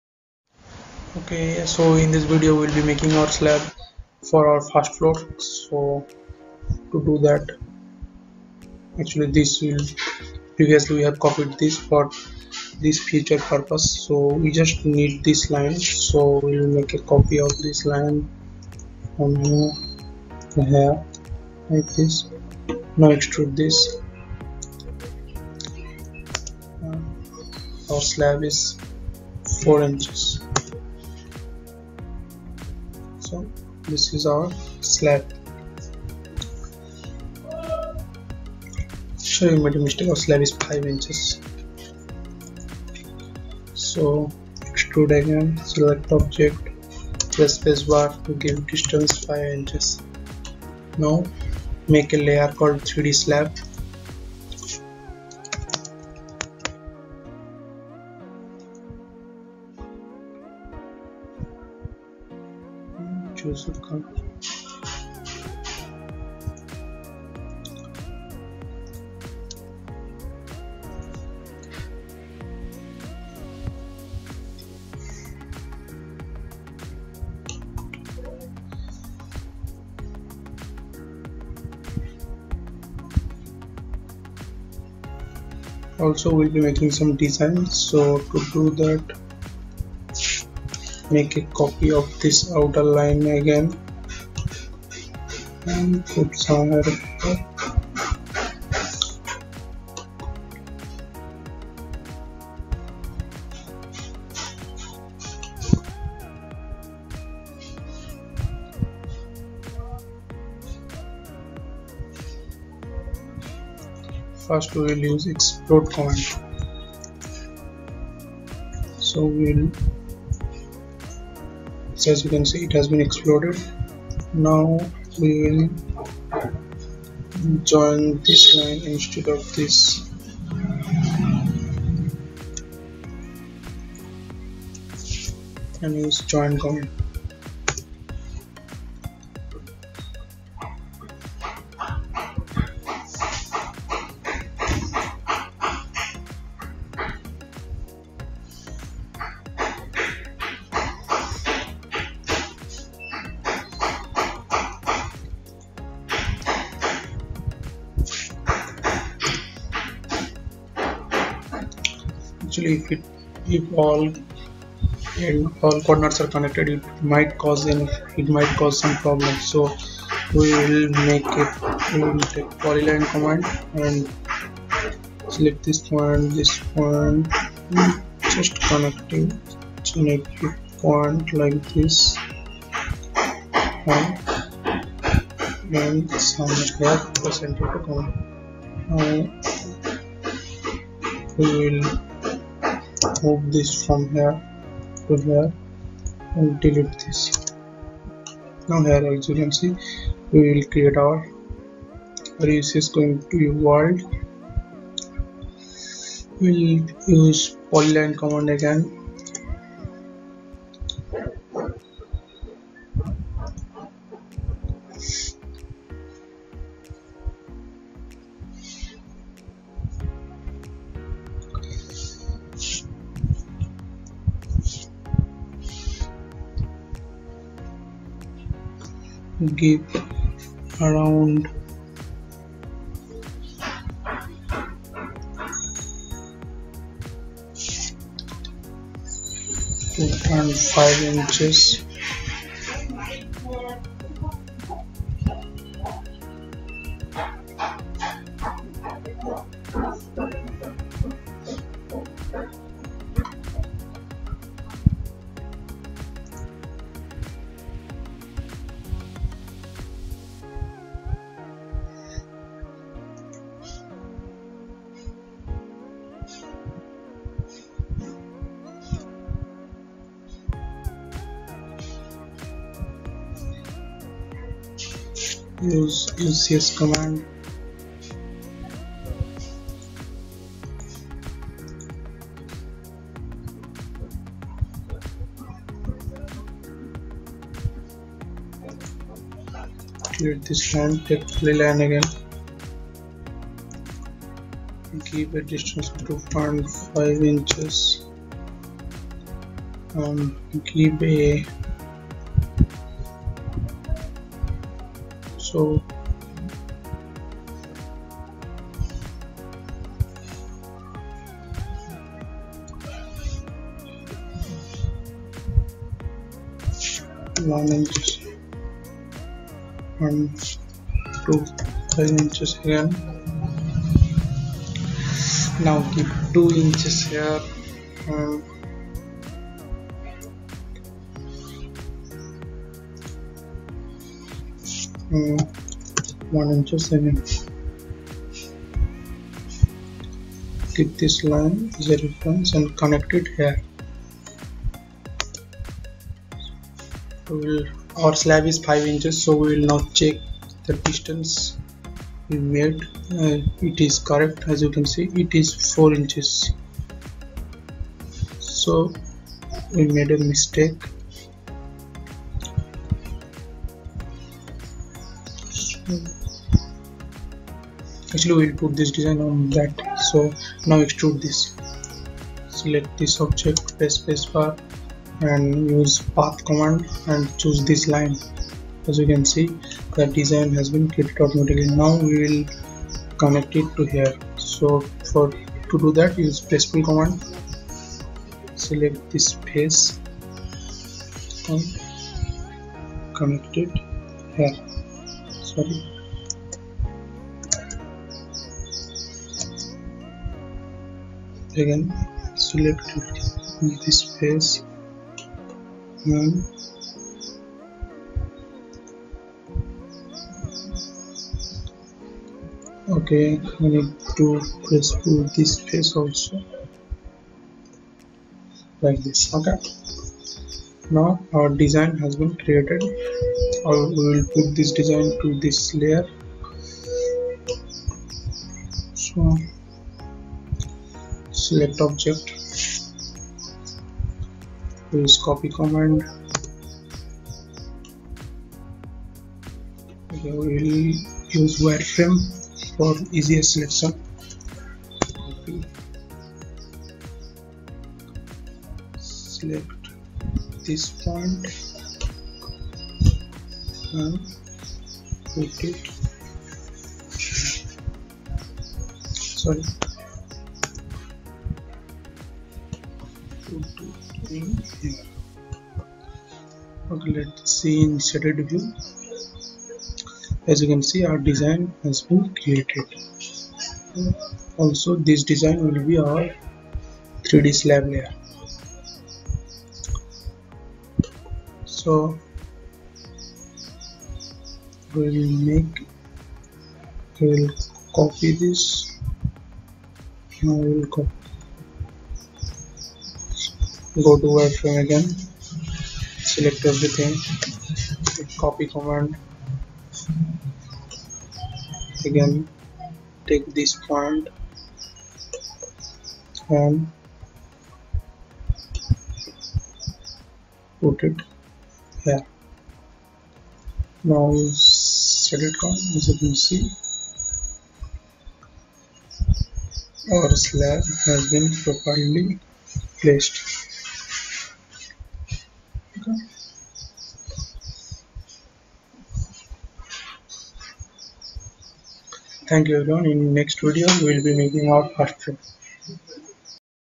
Making Ground Floor Roof Slab in 3D | AutoCAD 2D-3D Tutorial 2020 For Beginners
This is a tutorial of Making Ground Floor Roof Slab in 3D for beginners.
This a full tutorial/course for beginners with a 2 storey residential building project in 2d & 3d.
So keep watching & Learning.